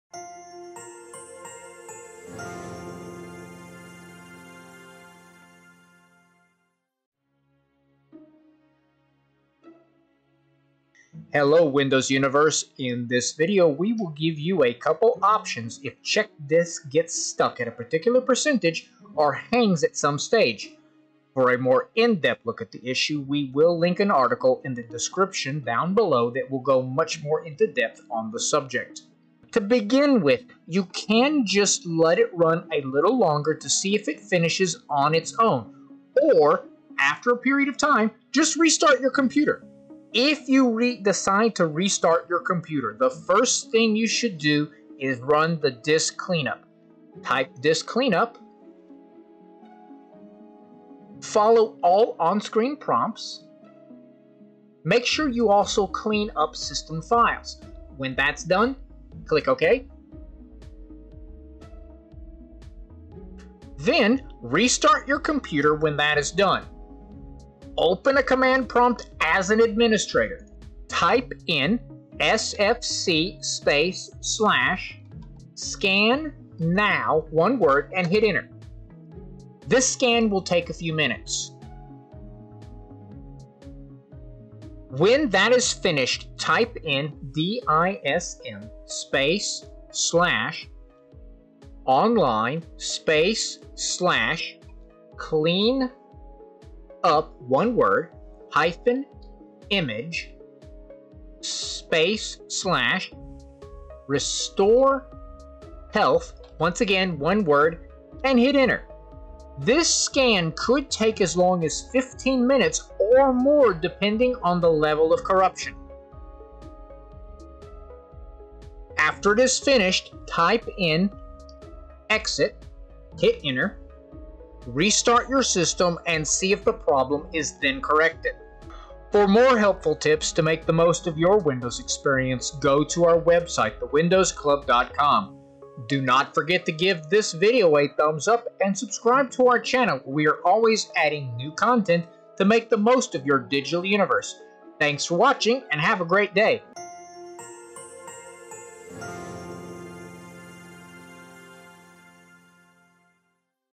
ChkDsk stuck at particular % or hangs at some Stage
If you find that Check Disk or ChkDsk is stuck at a particular percentage or hangs at some stage in Windows, then this post suggests some troubleshooting steps that may help you. It could be 10%, 12%, 27% or any such percentage. Again, it could be stages 2, 4, 5 or any such.

If ChkDsk hangs or is stuck, we suggest you try the following suggestions:

1] Wait for it to complete

2] Restart your PC & run Disk Cleanup, SFC, DISM